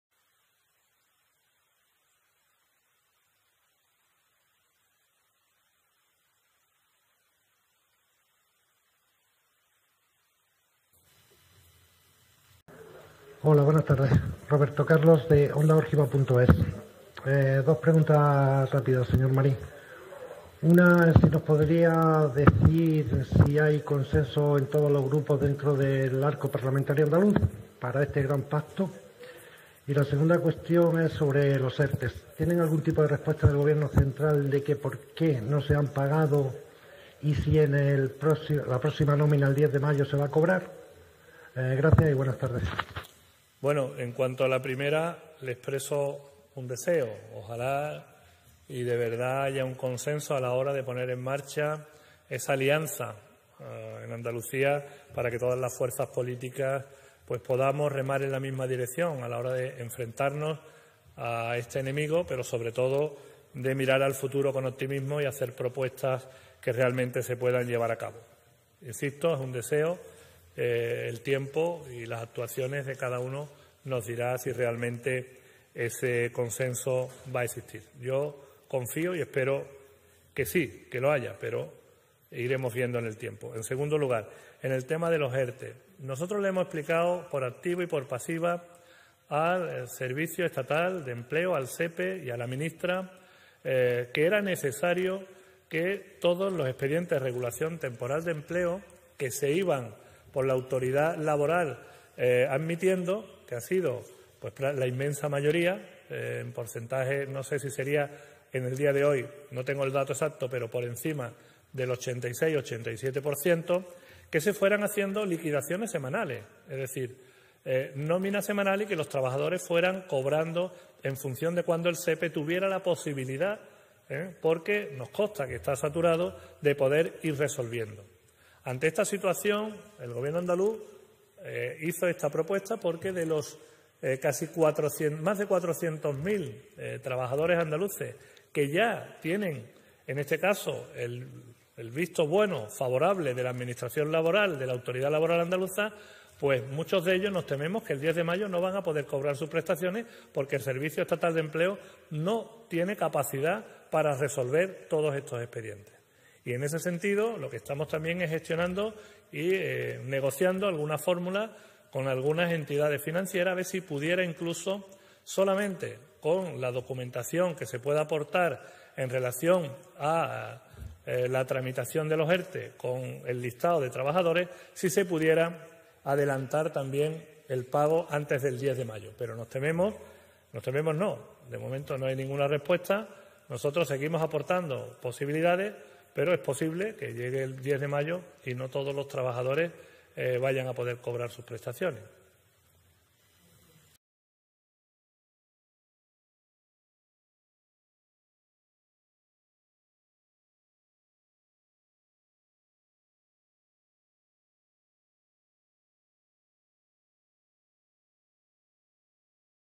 Rueda de prensa del vicepresidente de la Junta de Andalucía
28/04/2020
Juan Marín comparece ante los medios de comunicación tras la reunión del Comité de Seguimiento de la crisis sanitaria por el COVID-19.